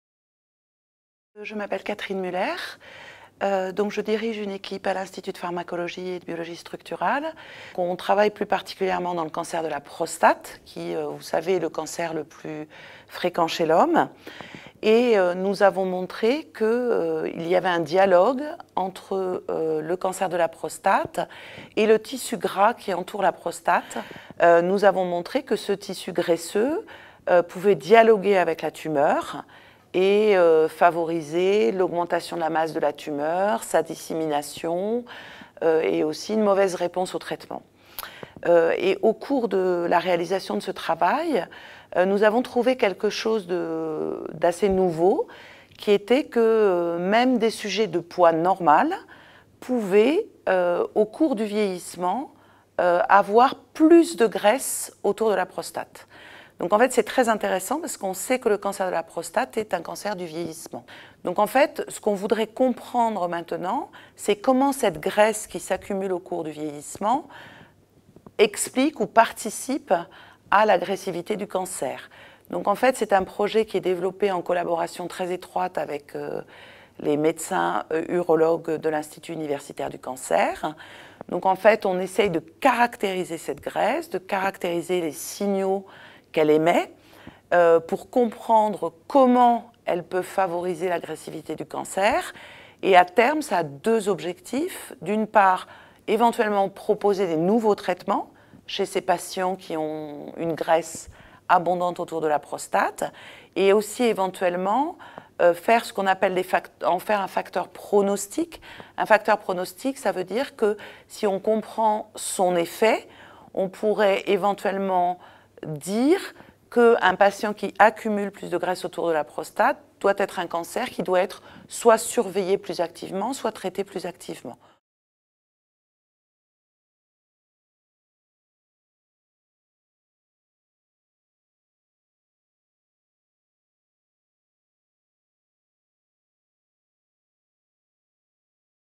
Fondation Toulouse Cancer : Professeur Catherine Müller-Staumont
Le Professeur Catherine Müller-Staumont, chef d’équipe «Microenvironnement, Cancer et Adipocytes» et Responsable du Département «Biologie du cancer» à l'Institut de Pharmacologie et de Biologie Structurale, mène un projet de recherche contre le cancer de la prostate.

Merci à l'ISPRA pour la réalisation de cette vidéo.